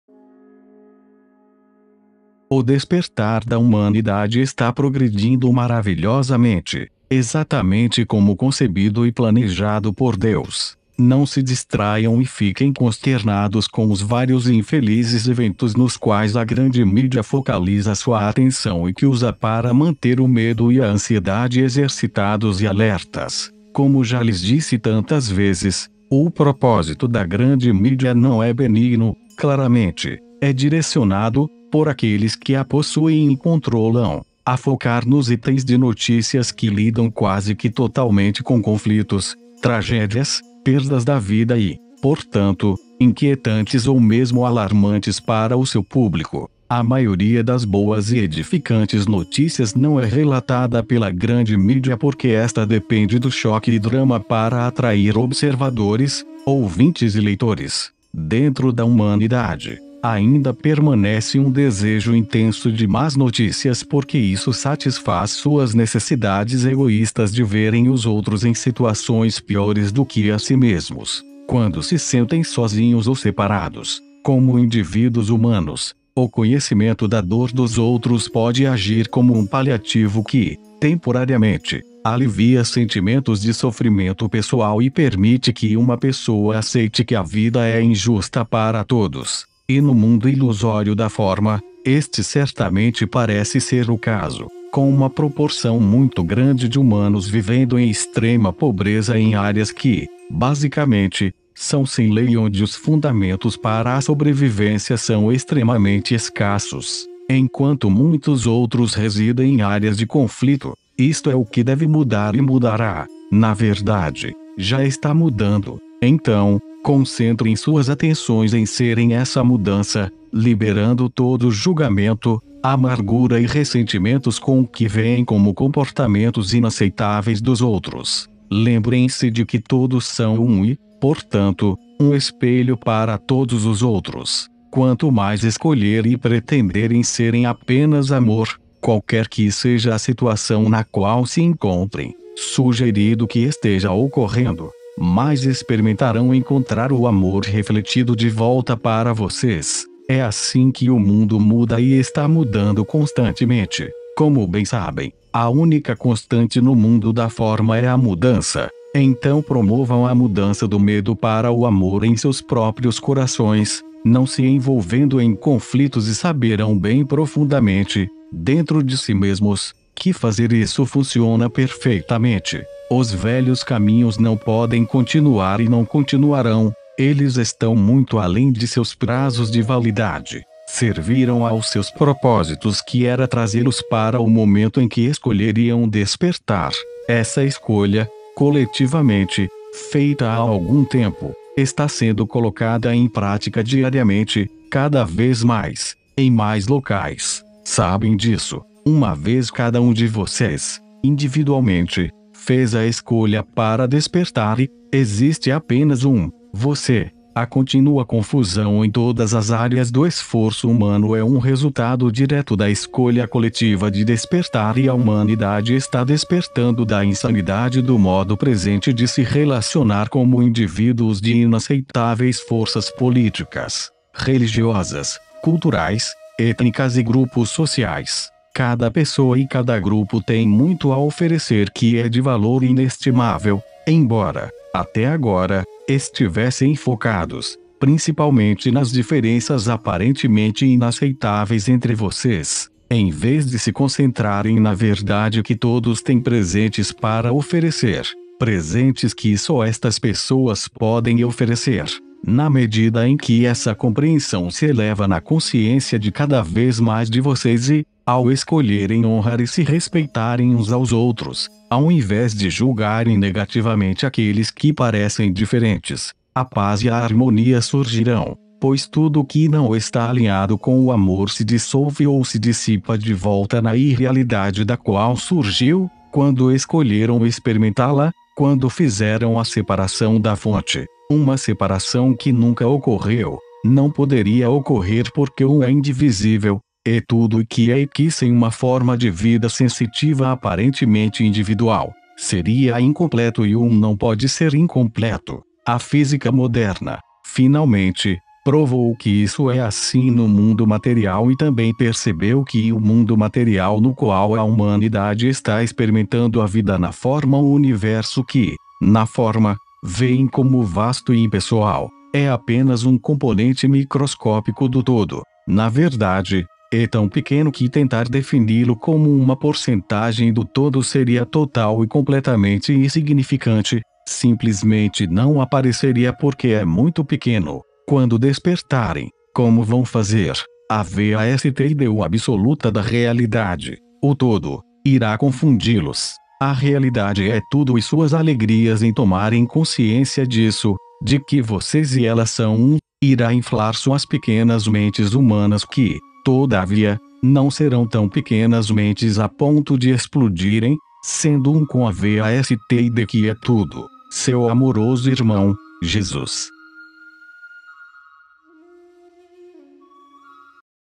JESUS - "A SEPARAÇÃO NUNCA ACONTECEU, NÃO PODERIA ACONTECER PORQUE O UM É INDIVISÍVEL"
Esta é uma narração da mensagem de   JESUS , postada em texto no blog Sementes das Estrelas.